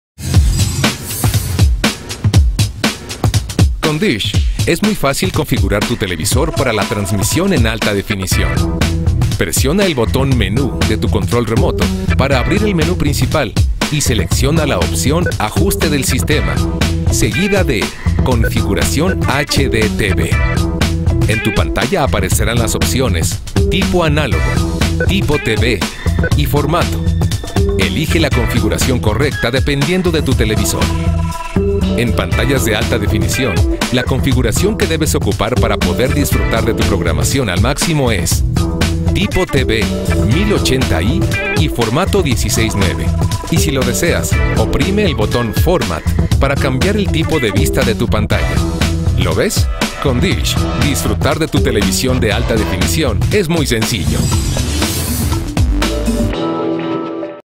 Configura correctamente tu equipo HD
Si tienes un equipo HD configúralo de esta manera para disfrutar al máximo tu experiencia en alta definición.